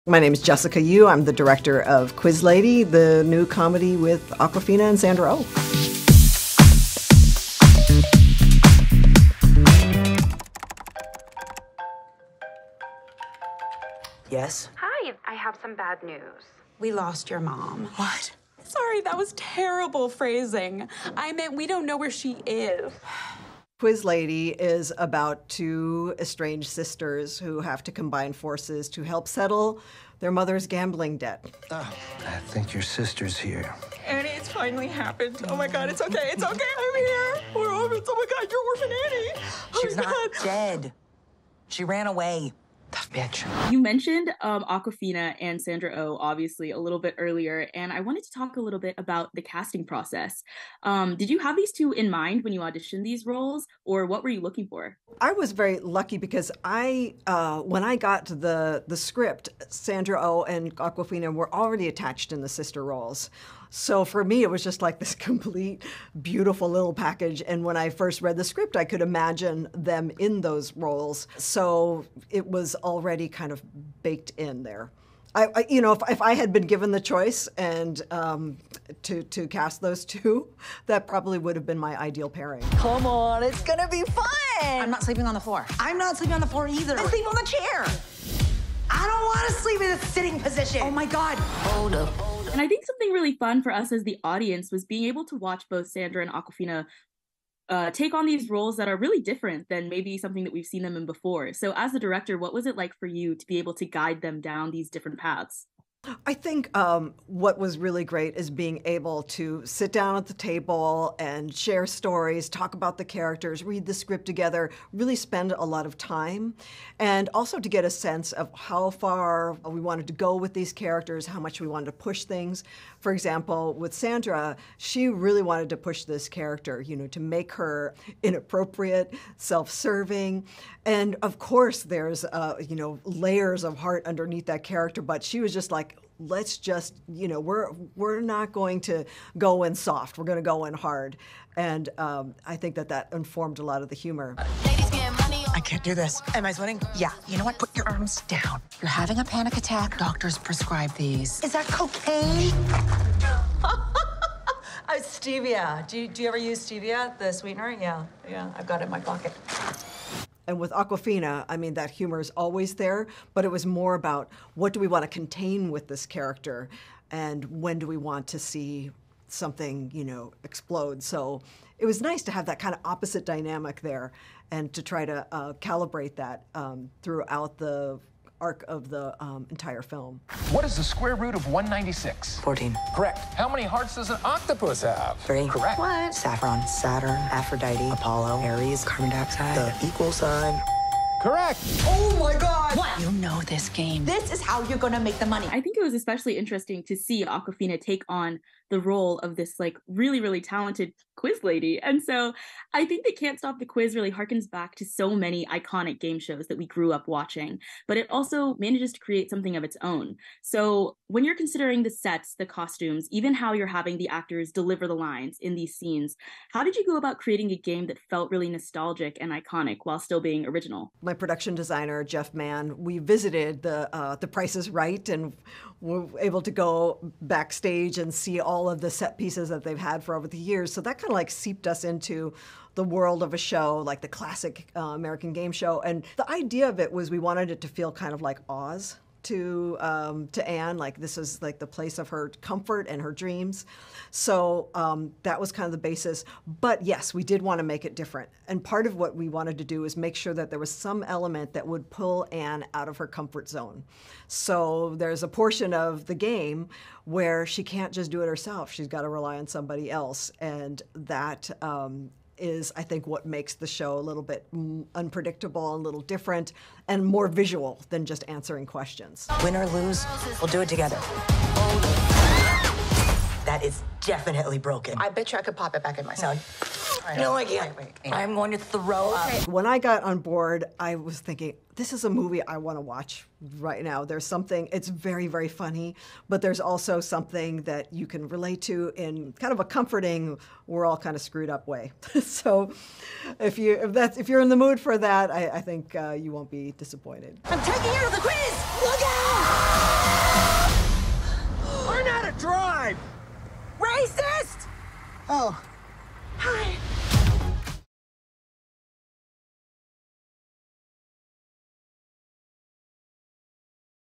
“Quiz Lady” Director Jessica Yu Talks Awkwafina and Sandra Oh’s Sister Act
Buzz in for our latest interview with "Quiz Lady" director Jessica Yu as she shares what it was like directing stars Awkwafina and Sandra Oh, how she and the production team created the atmosphere for "Can't Stop the Quiz" and more!

Interviewer: Emily Chung
Video Editor: Francis Ray